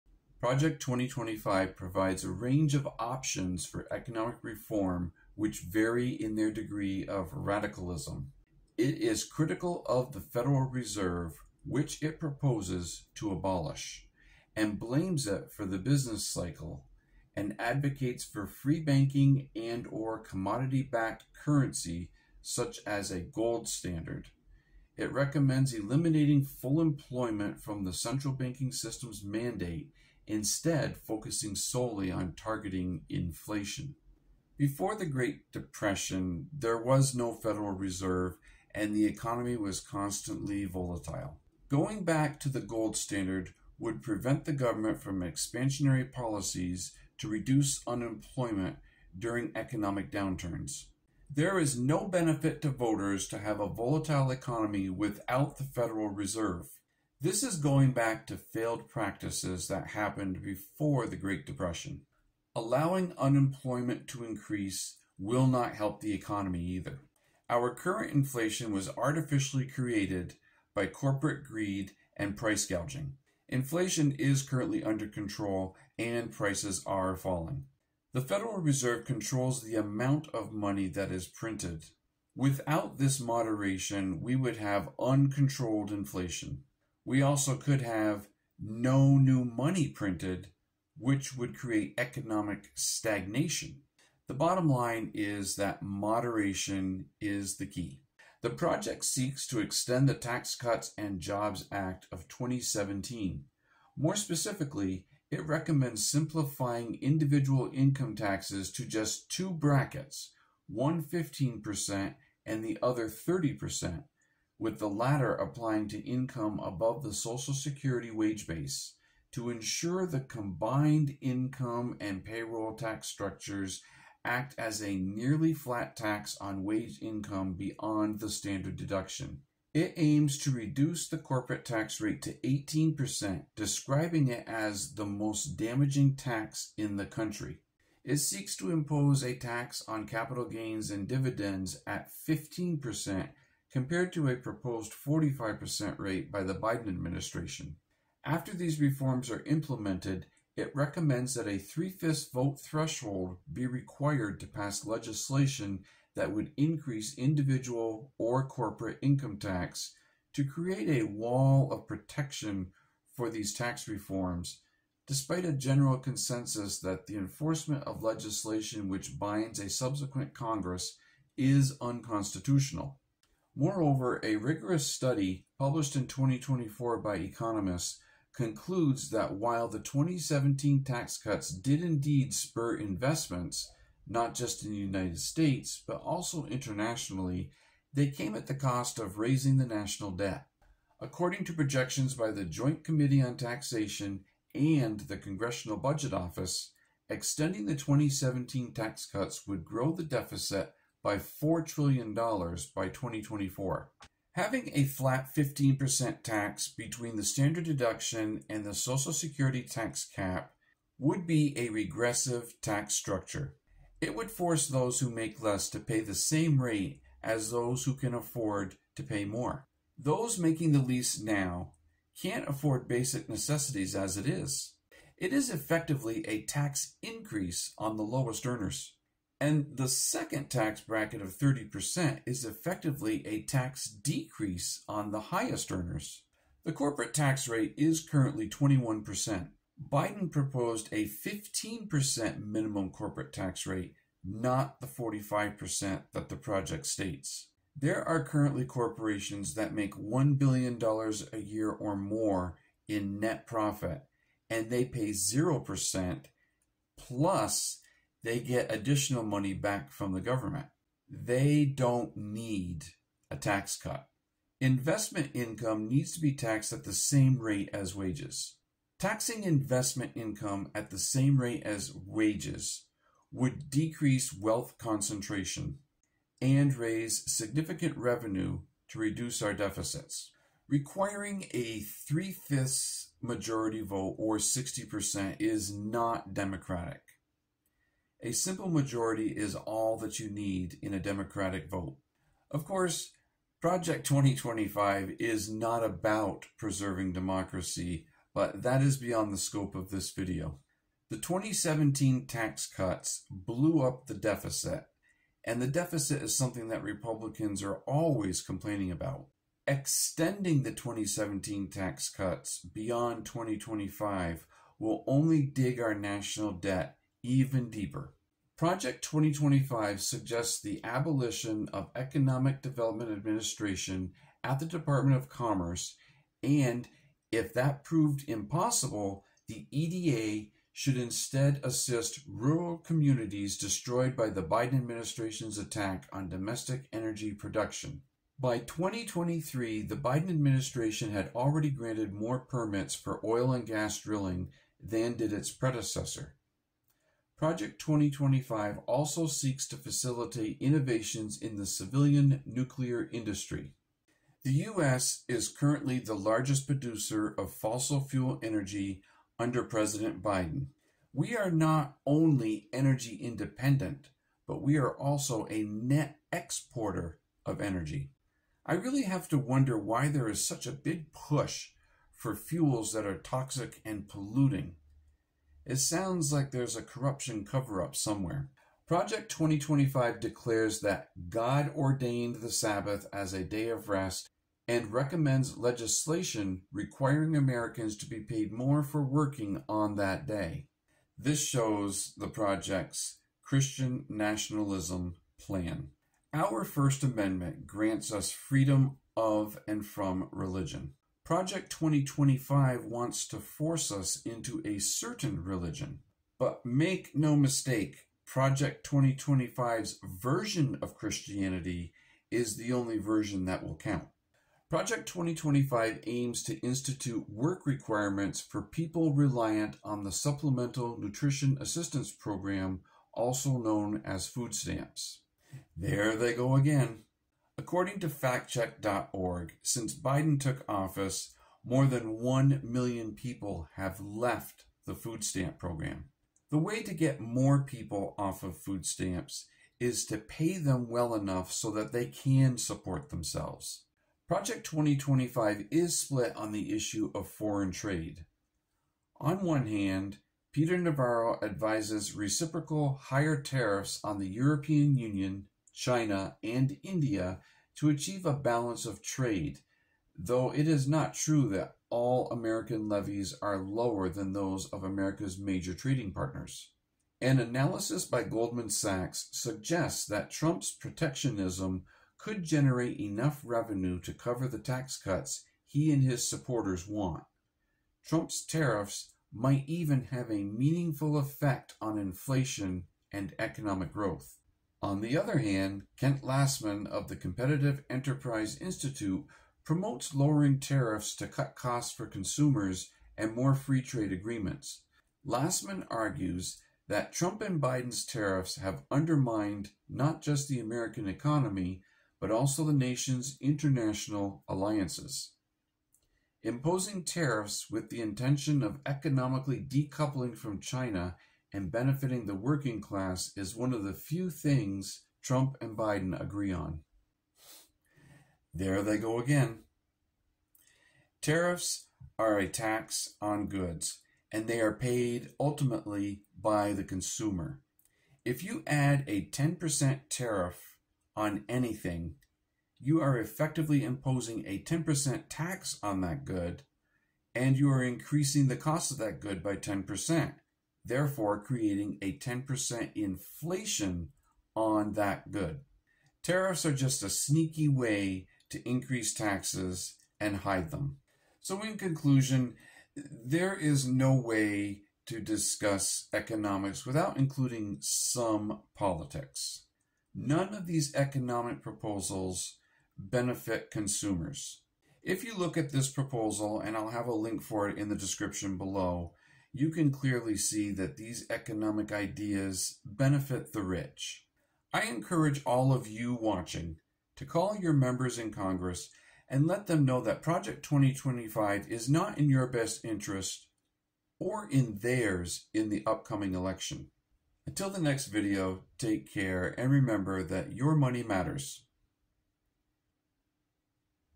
Why You Can't Ignore Project 2025's Impact 🔥
Project 2025 has many flaws that will affect the vast majority negatively.

Project 2025 seeks to increase taxes for the lower income earners and the middle class while cutting taxes further for the rich and corporations.  The economic impact will devastate the less well off.

Project 2025 seeks to ramp up fossil fuel and nuclear energy when we are already energy independent.  It also seeks to abolish the Economic Development Administration.

Project 2025 seeks to violate our First Amendment right of freedom of religion and push a Christian nation on us.

Project 2025 seeks to punish the poor who are on food stamps instead of ensuring that people are paid fairly for their work.

Finally, Project 2025 seeks to increase the cost of items we buy by imposing more tariffs, which will create more inflation.

📖Chapters:
00:00 Intro
01:37 Objectives
05:18 Suggestions
06:18 Religion
06:54 Food Stamps
07:17 Tariffs